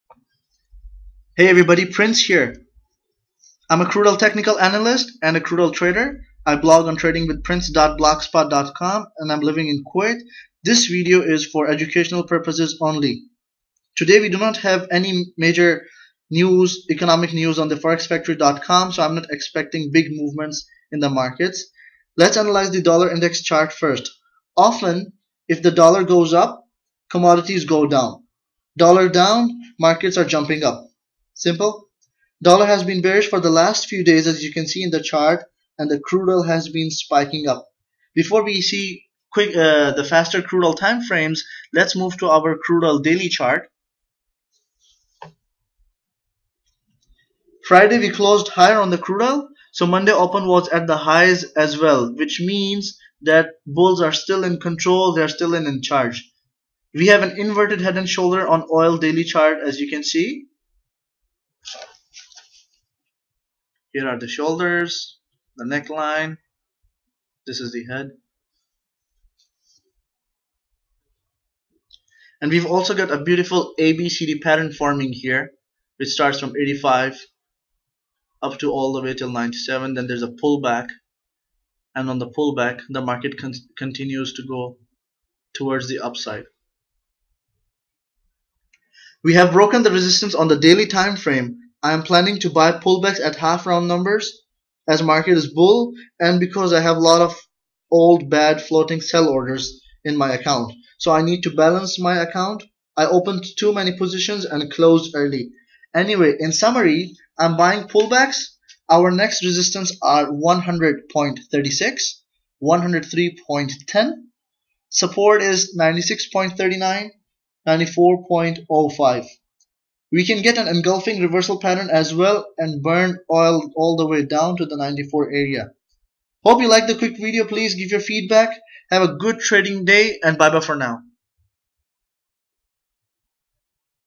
Trading crude oil - 17th June 2013 Analysis
Quick crude oil review. Buying the pull backs upto 100.36